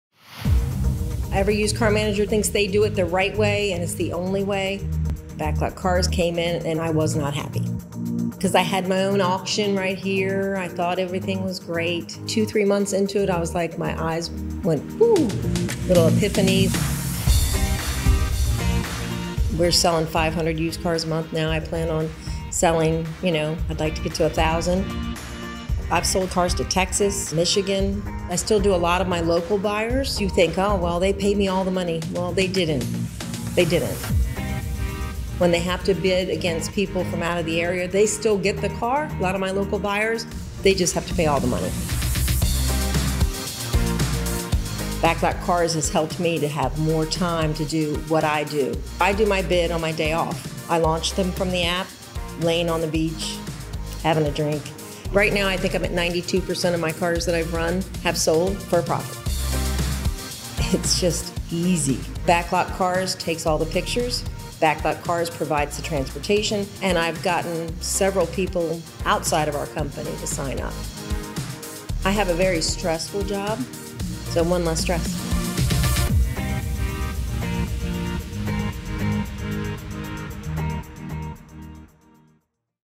Pohanka Automotive Sells Hundreds More Vehicles with BacklotCars
Don’t settle for just selling to local buyers.

BacklotCars is an online dealer-to-dealer platform that lets you sell trades to used car dealers nationwide. Sell on your schedule, 24 hours a day, 7 days a week.

For more dealer testimonials and user experiences, check out our Seller Playlist on YouTube